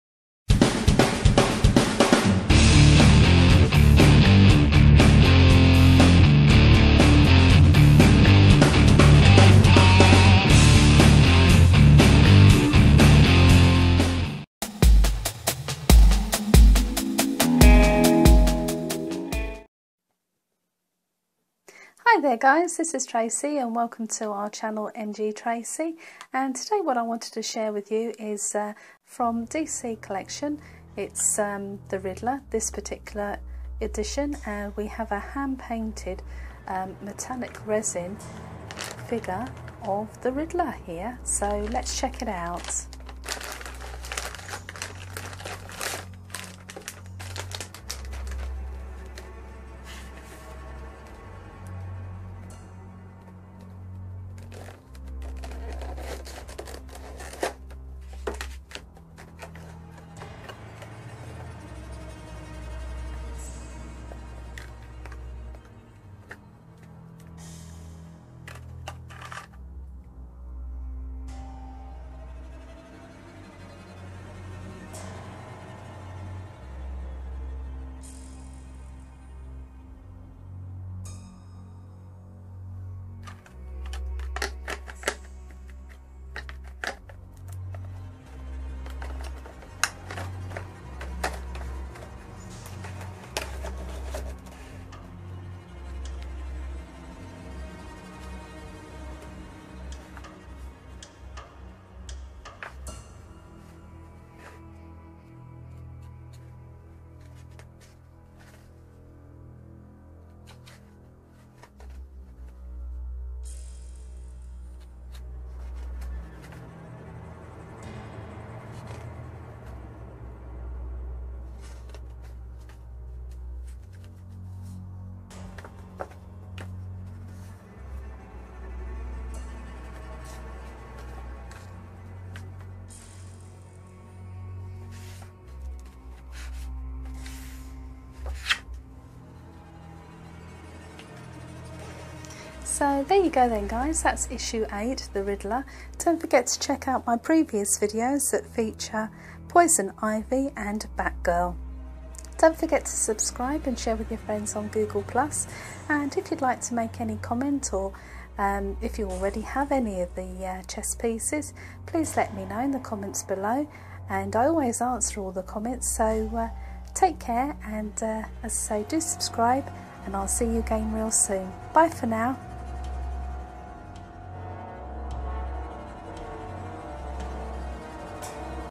Opening of DC Comics Chess Collection Issue 8 with hand painted THE RIDDLER
Opening of DC Comics Chess Collection Issue 8